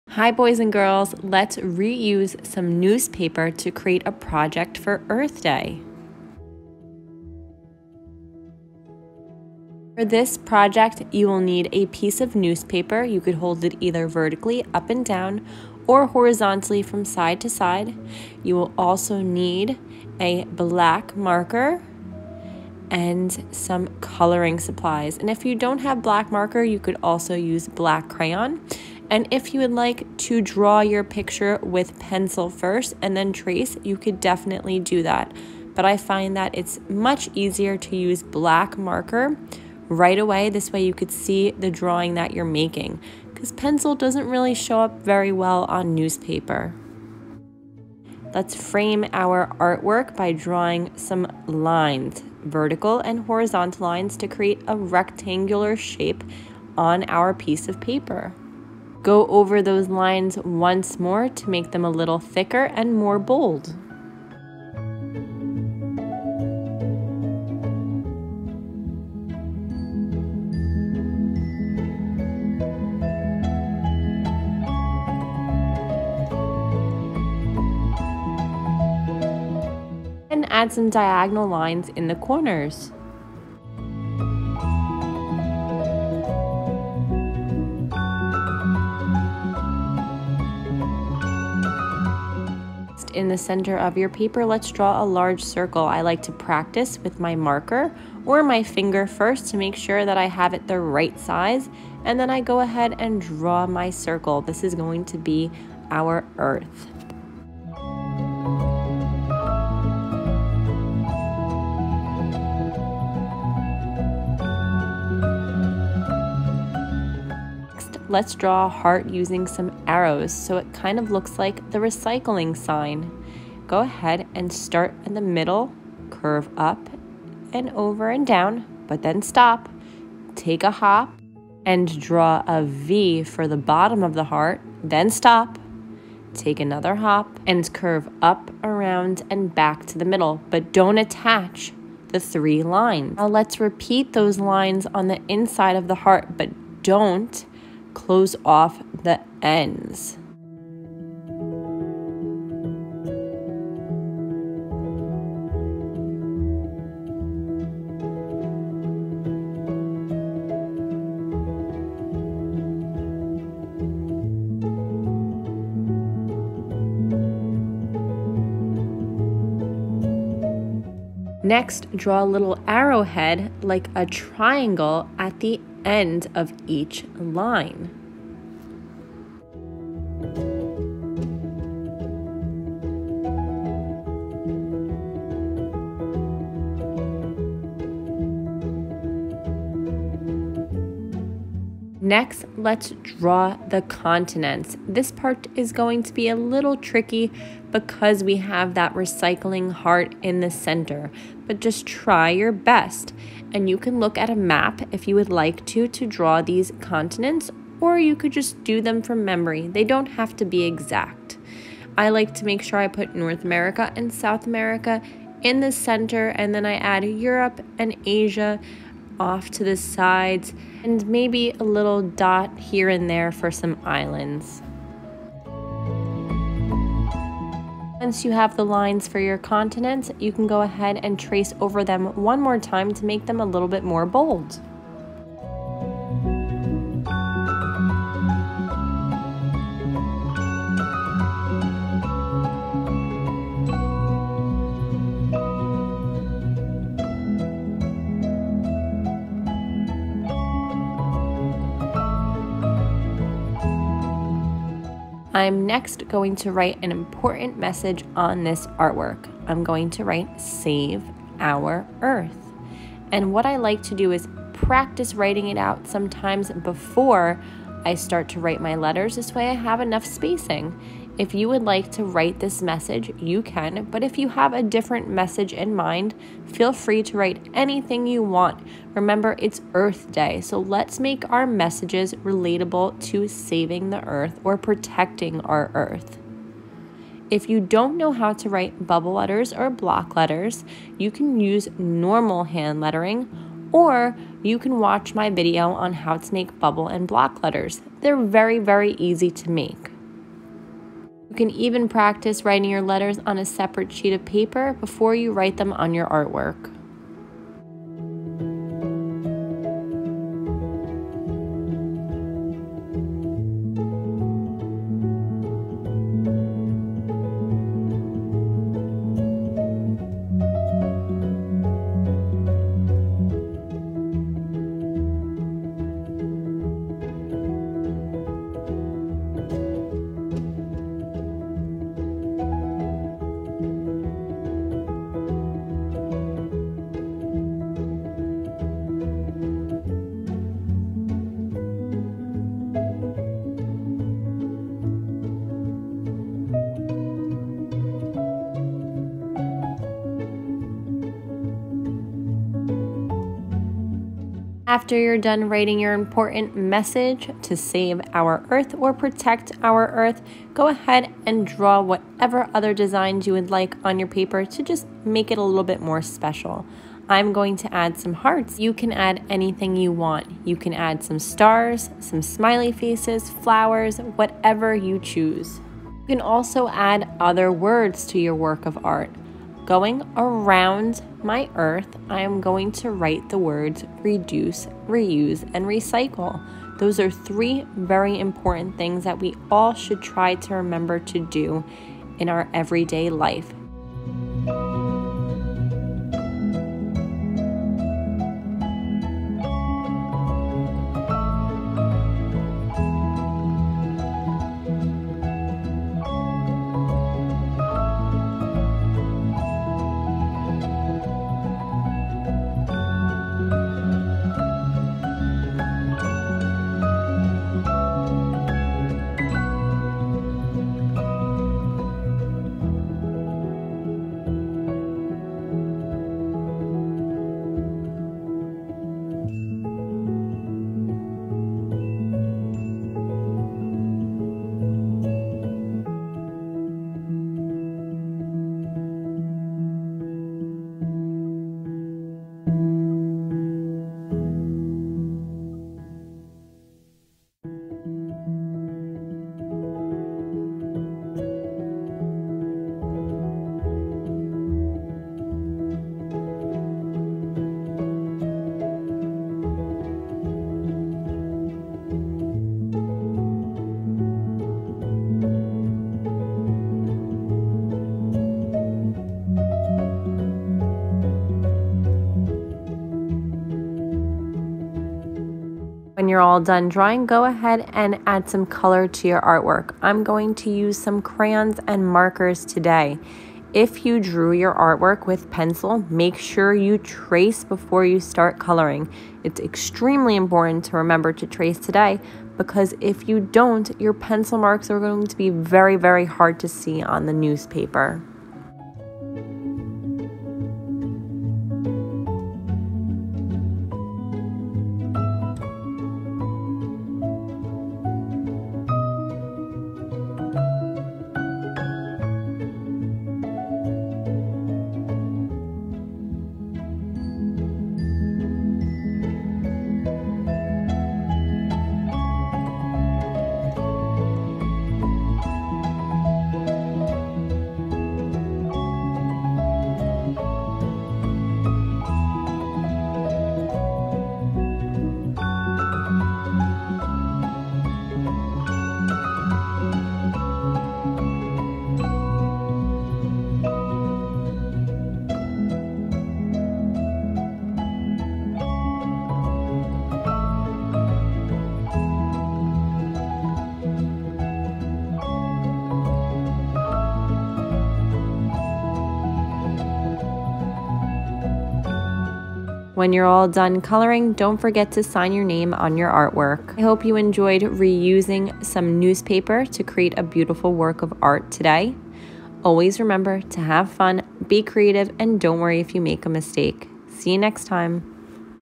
Earth Day!
Let’s reuse some newspaper to create a beautiful work fo art for Earth Day!